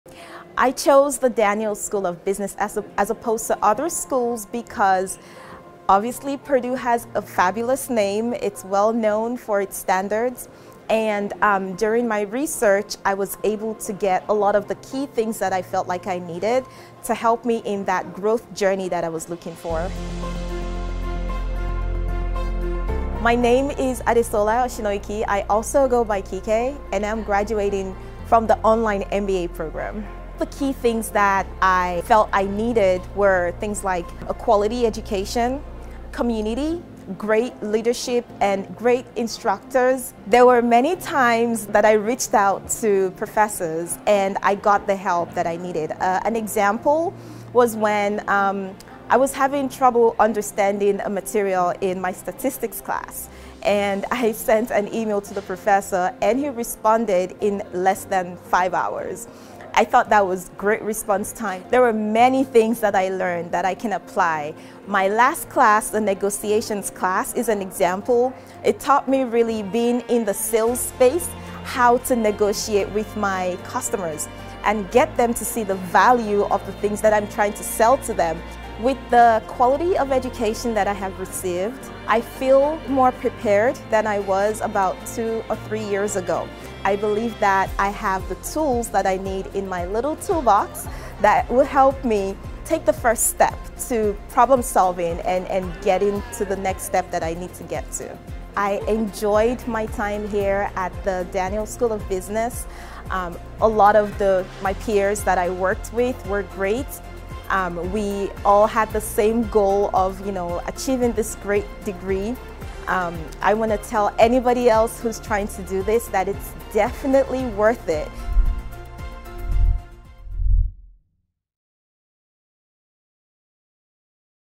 My Personal Growth Through Purdue Daniels Online MBA
Hear Kike share about her experience earning an Online MBA through the Mitch Daniels School of Business at Purdue University.

To learn more about the Mitch Daniels School of Business,